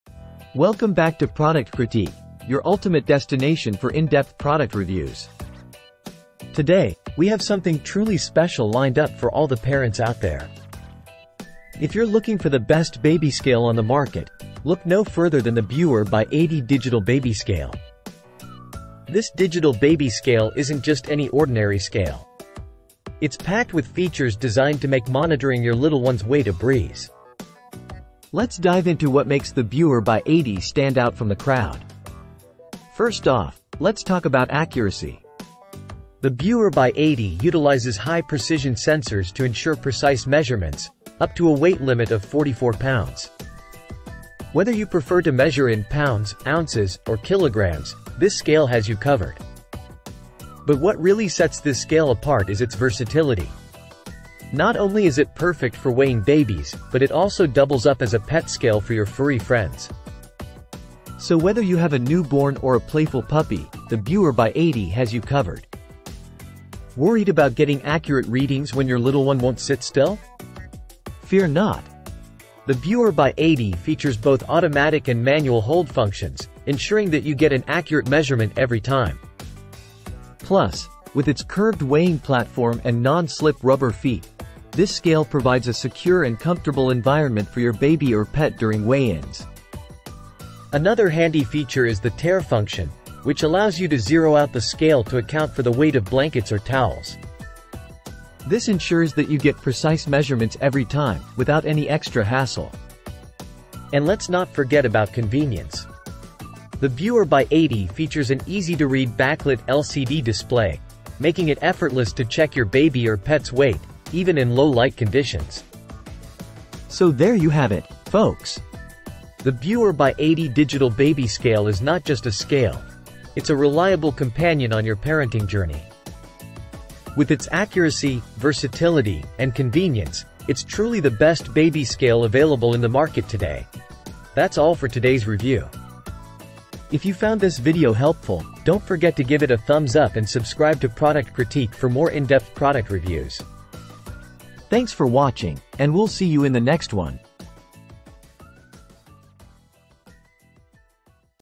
👶 Beurer BY80 Digital Baby Scale | Best Beurer Baby Scale 🍼
Amazon Product Name : Beurer BY80 Digital Baby Scale

Monitor your little one's growth with confidence using the Best Beurer Baby Scale, a reliable and accurate tool for tracking weight milestones from infancy to toddlerhood. Designed with precision and ease of use in mind, this scale features a comfortable, secure platform for your baby and provides precise measurements in grams or ounces. With its sleek design and user-friendly interface, the Best Beurer Baby Scale is a must-have for parents seeking peace of mind and accurate data for their baby's development. Watch the video to learn why the Best Beurer Baby Scale is trusted by parents worldwide for its quality and reliability.